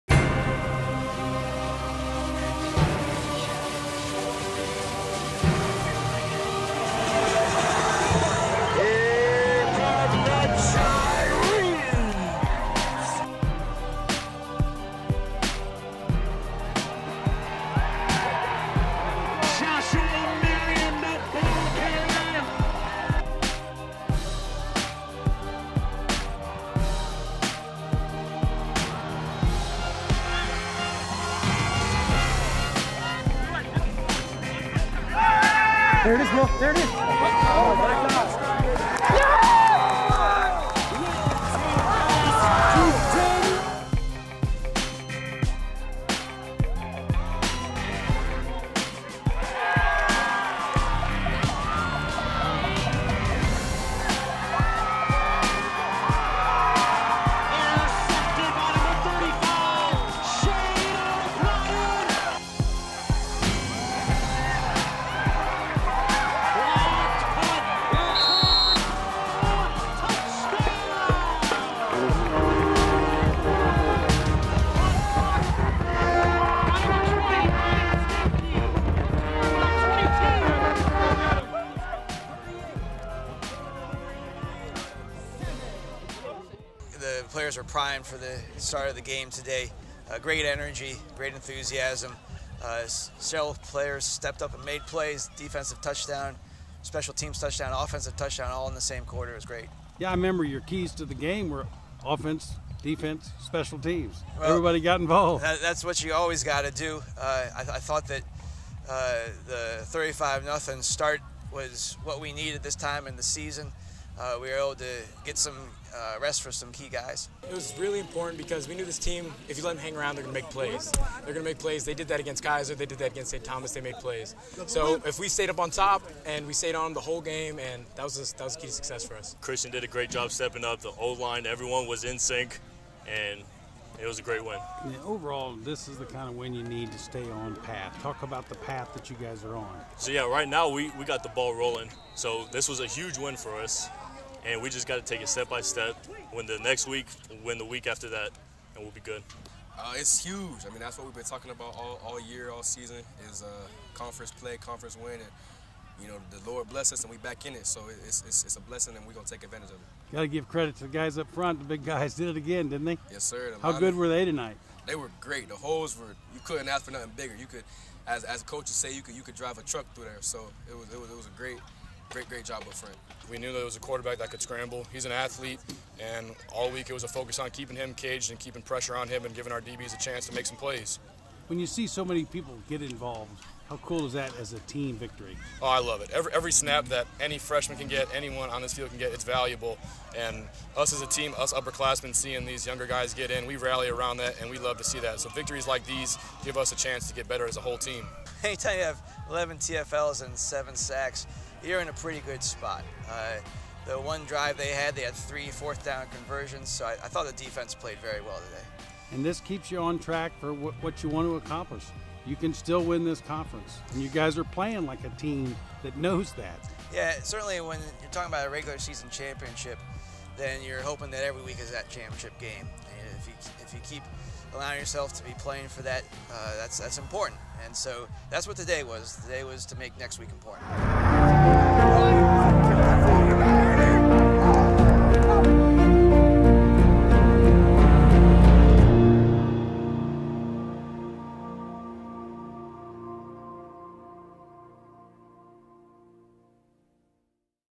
Ave Maria Highlights vs Florida Mem
Joe Patterson's Gyrenes improve to 3-1 in the conference race and stay in the chase for a title with a 38-7 win over Florida Memorial. Take a look at the highlights and interviews from Saturday night.